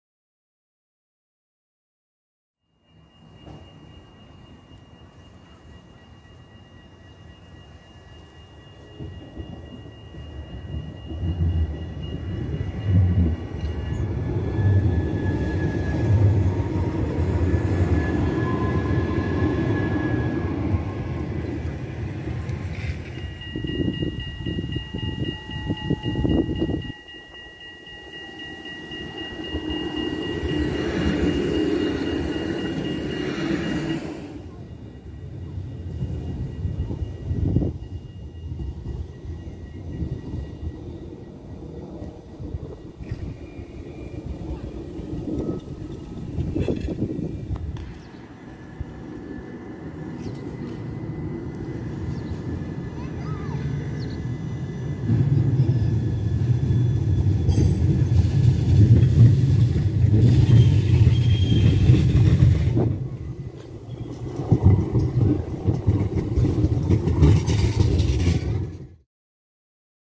Dutch trams #1 Ypenburg
This video is showing two lines placed in Ypenburg in The Netherlands.